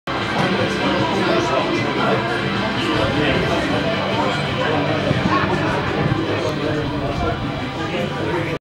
Mark - Assistant Referee - FA Vase - Newport (IoW) vs Calne Town - 20th Sept. 2020
Mark Bradley appointed Assistant Referee for the 1st time in a FA Vase game on 20th September 2020.
Newport (Isle of Wight) vs Calne Town (Wiltshire)
Newport won 5-3 on pens after Extra Time.